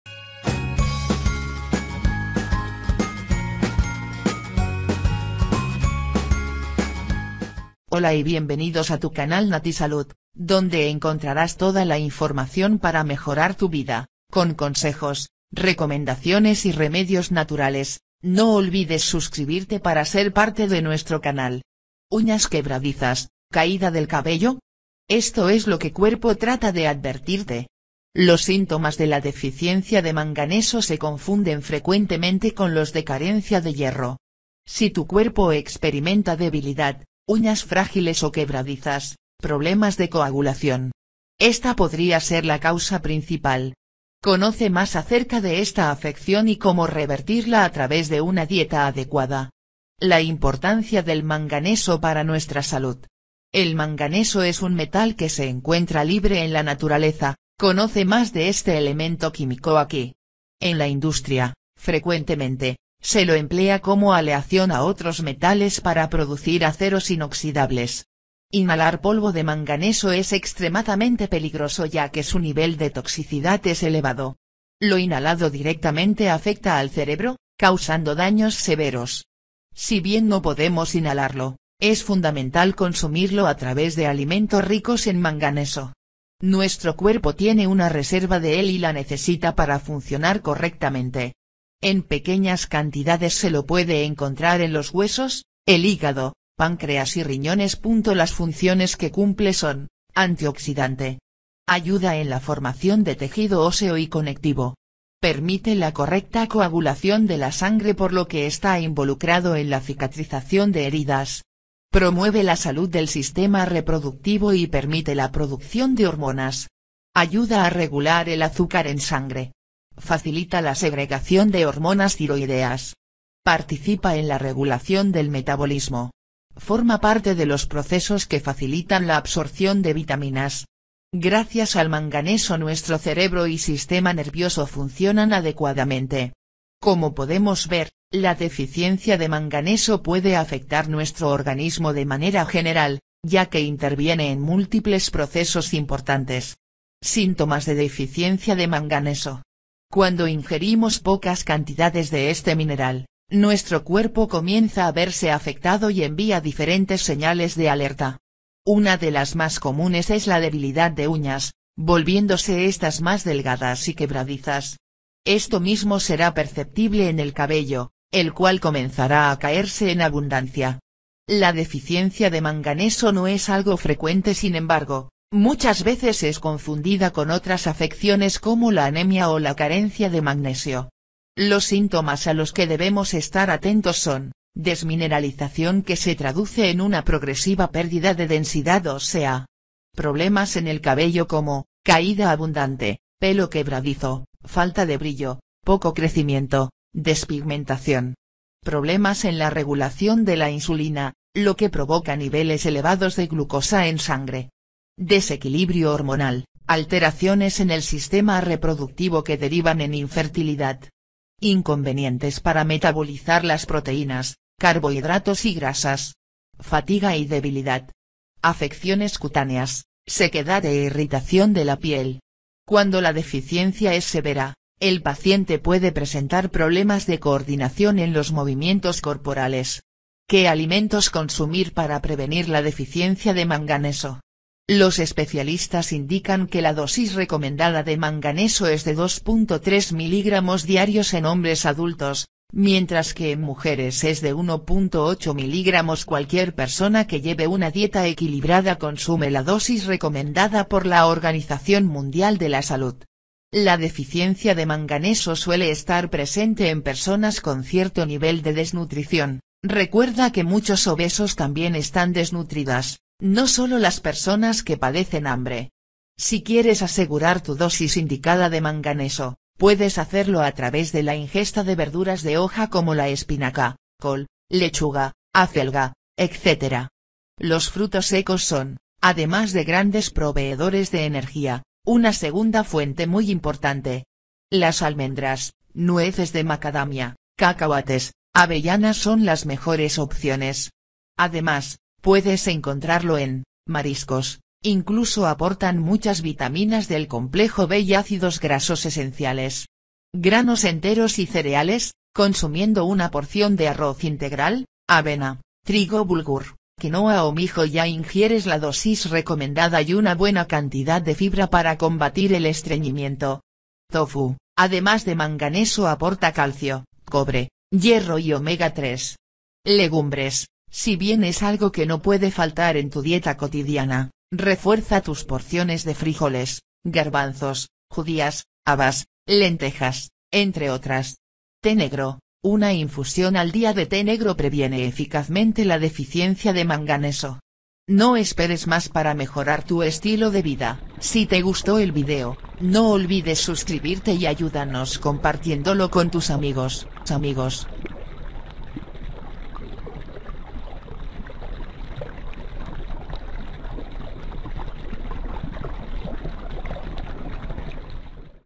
UÑAS QUEBRADIZAS, CAÍDA DEL CABELLO ESTO ES LO QUE CUERPO TRATA DE ADVERTIRTE…
UÑAS QUEBRADIZAS, CAÍDA DEL CABELLO ESTO ES LO QUE CUERPO TRATA DE ADVERTIRTE…Los síntomas de la deficiencia de manganeso se confunden frecuentemente con los de carencia de hierro. Si tu cuerpo experimenta debilidad, uñas frágiles o quebradizas, problemas de coagulación… esta podría ser la causa principal. Conoce más acerca de esta afección y cómo revertirla a través de una dieta adecuada.

LA IMPORTANCIA DEL MANGANESO PARA NUESTRA SALUD.
El manganeso es un metal que se encuentra libre en la naturaleza (conoce más de este elemento químico aquí). En la industria, frecuentemente, se lo emplea como aleación a otros metales para producir aceros inoxidables. Inhalar polvo de manganeso es extremadamente peligroso ya que su nivel de toxicidad es elevado. Lo inhalado directamente afecta al cerebro, causando daños severos.

Si bien no podemos inhalarlo, es fundamental consumirlo a través de alimentos ricos en manganeso. Nuestro cuerpo tiene una reserva de él y la necesita para funcionar correctamente. En pequeñas cantidades se lo puede encontrar en los huesos, el hígado, páncreas y riñones.Las funciones que cumple son:

Antioxidante.
Ayuda en la formación de tejido óseo y conectivo.
Permite la correcta coagulación de la sangre por lo que está involucrado en la cicatrización de heridas.
Promueve la salud del sistema reproductivo y permite la producción de hormonas.
Ayuda a regular el azúcar en sangre.
Facilita la segregación de hormonas tiroideas.
Participa en la regulación del metabolismo.
Forma parte de los procesos que facilitan la absorción de vitaminas.
Gracias al manganeso nuestro cerebro y sistema nervioso funcionan adecuadamente.
Como podemos ver, la deficiencia de manganeso puede afectar nuestro organismo de manera general, ya que interviene en múltiples procesos importantes.

SÍNTOMAS DE DEFICIENCIA DE MANGANESO.
Cuando ingerimos pocas cantidades de este mineral, nuestro cuerpo comienza a verse afectado y envía diferentes señales de alerta. Una de las más comunes es la debilidad de uñas, volviéndose estas más delgadas y quebradizas. Esto mismo será perceptible en el cabello, el cual comenzará a caerse en abundancia.

La deficiencia de manganeso no es algo frecuente sin embargo, muchas veces es confundida con otras afecciones como la anemia o la carencia de magnesio. Los síntomas a los que debemos estar atentos son:

Desmineralización que se traduce en una progresiva pérdida de densidad ósea.
Problemas en el cabello como: caída abundante, pelo quebradizo, falta de brillo, poco crecimiento, despigmentación.
Problemas en la regulación de la insulina, lo que provoca niveles elevados de glucosa en sangre.
Desequilibrio hormonal, alteraciones en el sistema reproductivo que derivan en infertilidad.
Inconvenientes para metabolizar las proteínas, carbohidratos y grasas.
Fatiga y debilidad.
Afecciones cutáneas, sequedad e irritación de la piel.
Cuando la deficiencia es severa, el paciente puede presentar problemas de coordinación en los movimientos corporales.
¿QUÉ ALIMENTOS CONSUMIR PARA PREVENIR LA DEFICIENCIA DE MANGANESO?
Los especialistas indican que la dosis recomendada de manganeso es de 2.3 mg diarios en hombres adultos, mientras que en mujeres es de 1.8 mg. Cualquier persona que lleve una dieta equilibrada consume la dosis recomendada por la Organización Mundial de la Salud. La deficiencia de manganeso suele estar presente en personas con cierto nivel de desnutrición (recuerda que muchos obesos también están desnutridas, no sólo las personas que padecen hambre).

Si quieres asegurar tu dosis indicada de manganeso, puedes hacerlo a través de la ingesta de verduras de hoja como la espinaca, col, lechuga, acelga, etc. Los frutos secos son, además de grandes proveedores de energía, una segunda fuente muy importante. Las almendras, nueces de macadamia, cacahuates, avellanas son las mejores opciones.

Además, puedes encontrarlo en:

Mariscos: Incluso aportan muchas vitaminas del complejo B y ácidos grasos esenciales.
Granos enteros y cereales: Consumiendo una porción de arroz integral, avena, trigo bulgur, quinoa o mijo ya ingieres la dosis recomendada y una buena cantidad de fibra para combatir el estreñimiento.
Tofu: Además de manganeso aporta calcio, cobre, hierro y Omega 3.
Legumbres: Si bien es algo que no puede faltar en tu dieta cotidiana, refuerza tus porciones de frijoles, garbanzos, judías, habas, lentejas, entre otras.
Té negro: Una infusión al día de té negro previene eficazmente la deficiencia de manganeso.